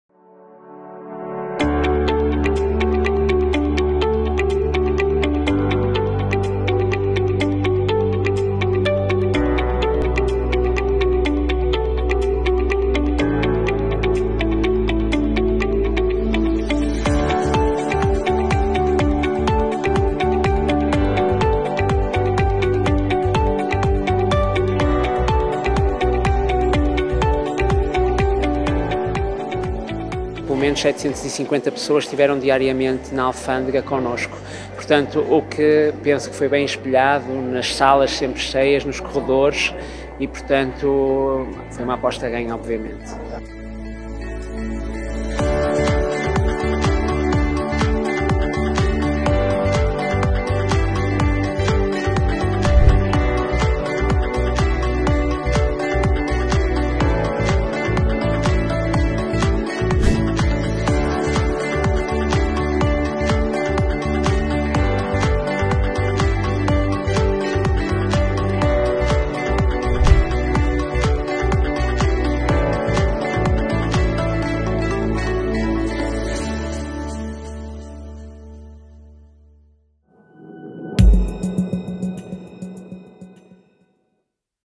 Uma viagem pelos melhores momentos do 19.º Congresso Nacional de Oncologia
Com os olhos postos no futuro, este ano, a Sociedade Portuguesa de Oncologia celebra os seus 40 anos e o 19.º Congresso Nacional de Oncologia foi o mote de arranque para este ano que promete ser muito especial. Em 18 de novembro, o Centro de Congressos da Alfândega do Porto fechou portas, após três dias de partilha de conhecimento e experiências multidisciplinares.